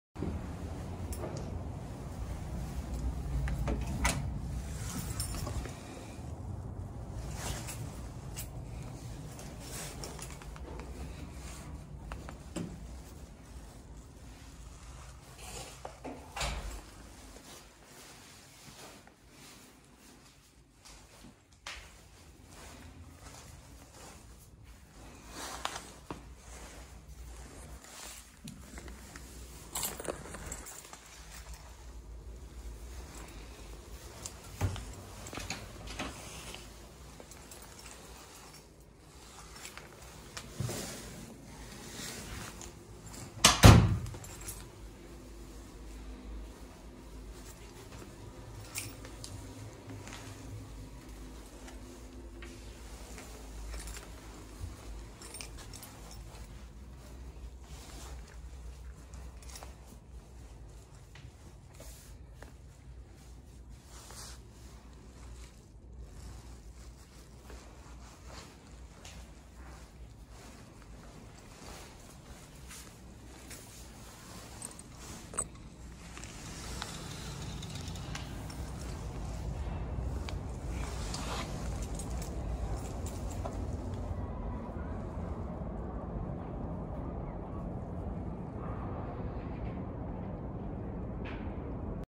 Seishin Minami Heights, 2LDK, 65sqm, 12F/14, south facing, , ｜ur apartment｜ur housing｜UR JKK賃貸
1-3-12※※, 130+5K
Seishin Minami Heights, 2LDK, 65sqm, 12F/14, south facing, , with AC and stove